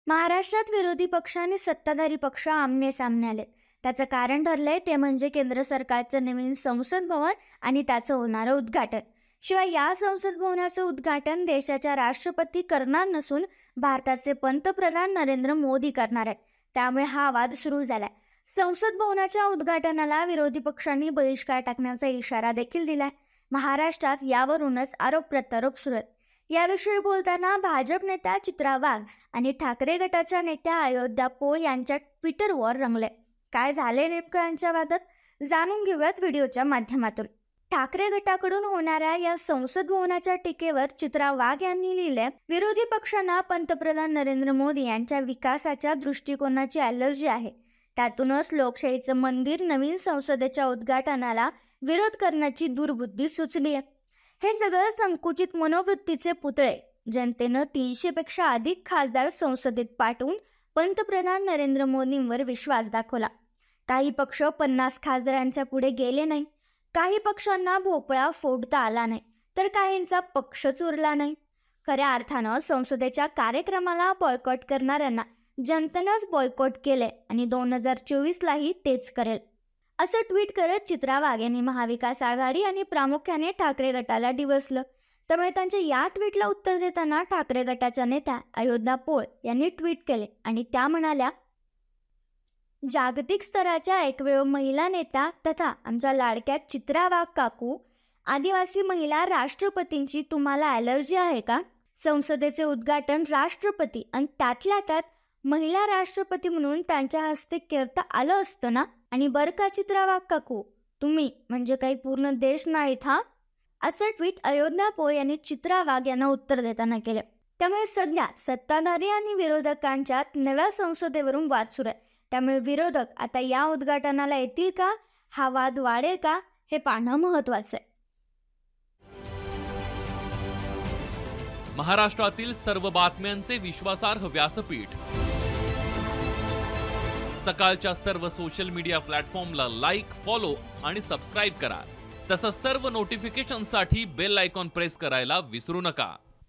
Chitra Wagh आणि Ayodhya Poul या दोघींमध्ये नव्या संसद भवनावरुन आरोप-प्रत्यारोप
नव्या संसदभवनावरुन  महाराष्ट्रात आरोप-प्रत्योरोप सुरु आहेत. याचविषयी बोलताना भाजप नेत्या चित्रा वाघ आणि शिवसेनेच्या नेत्या अयोध्या पोठ यांच्या ट्विटर वॉर रंगल आहे. काय झालयं नेमकं यांच्यात वादात जाणून घेऊयात व्हिडीओच्या माध्यमातून.